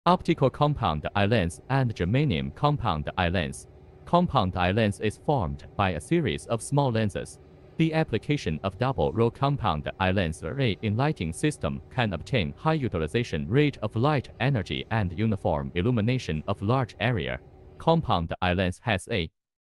Compound eye lens
Compound eye lens arrays usually come in pairs and, together with condenser lenses, provide a uniform irradiance distribution for the illuminated plane. The first compound eye lens array is usually called the objective lens array and the second compound eye lens array along the optical axis is called the field lens array. In this example we first consider the objective array. The function of the objective lens array is similar to that of the objective lens in a camera. It is used to image the object, or image the light source in this case on the rear focal plane of the objective lens array. By default, the light source emits parallel light and converges on the rear focal plane after passing through the objective lens array.